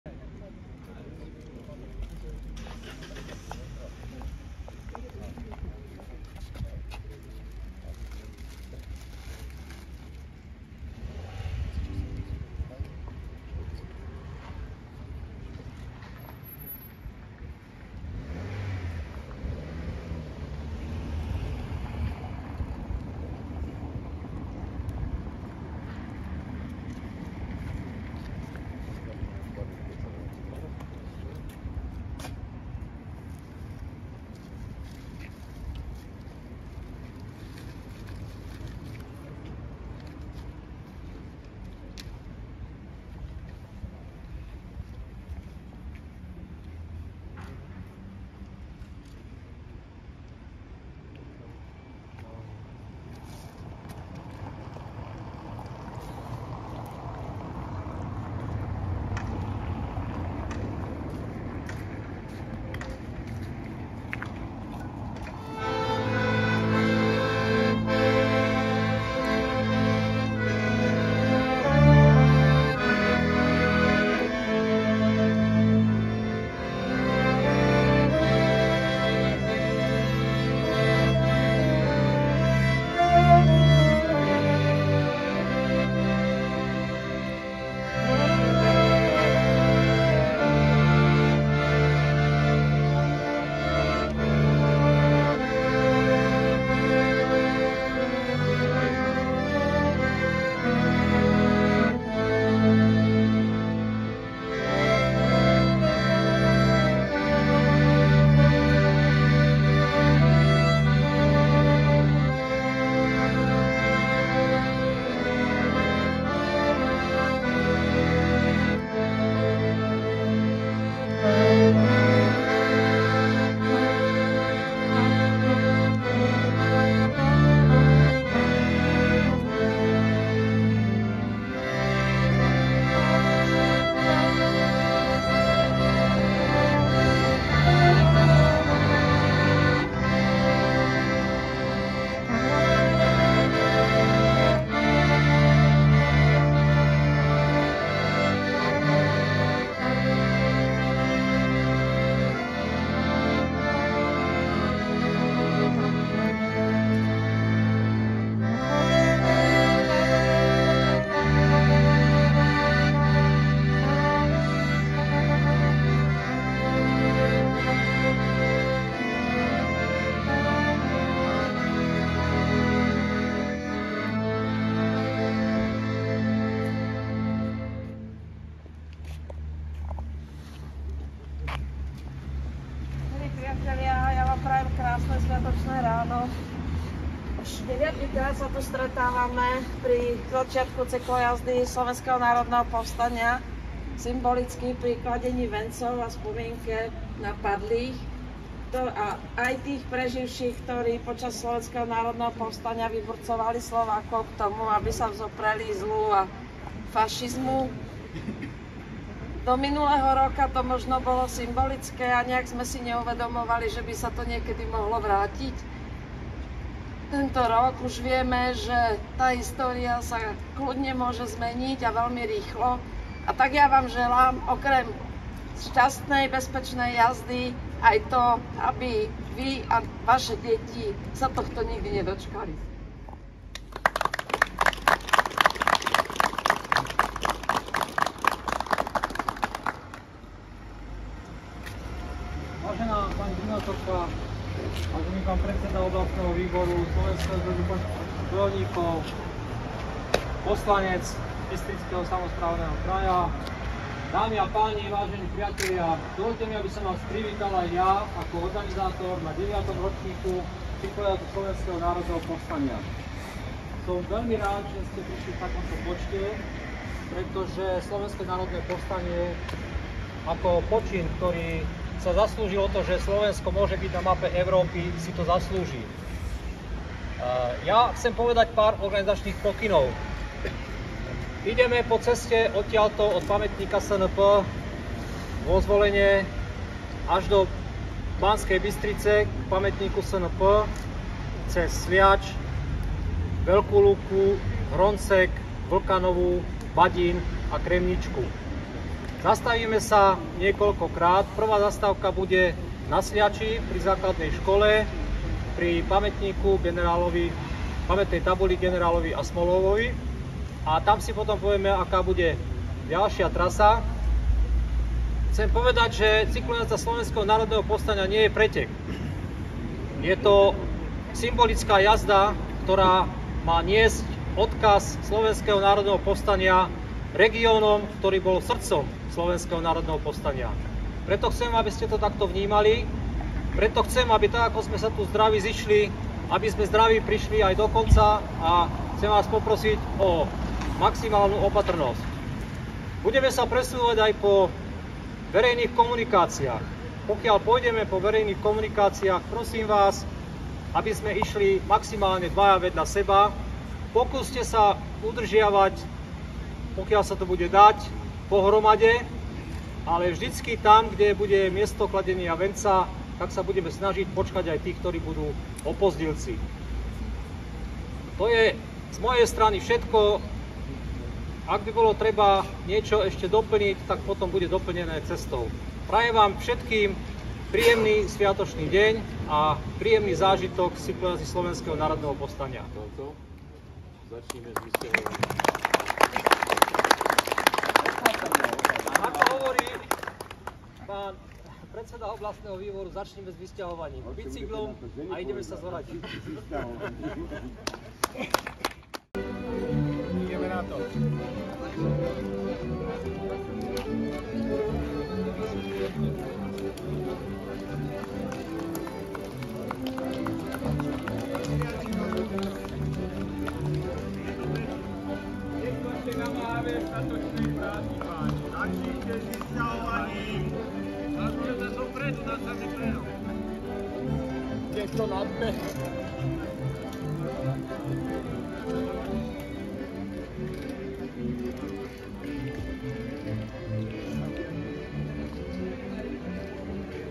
9. ročník Cyklojazdy SNP 2022
V pondelok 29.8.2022 začalo pri pamätníku SNP (Valaška) podujatie pod názvom 9. ročník Cyklojazdy SNP zo Zvolena do Banskej Bystrice.

V úvode podujatia sa kládli vence a pár viet povedala primátorka mesta Zvolen Lenka Balkovičová a následne potom organizátor podujatia Ing. Pavel Marko.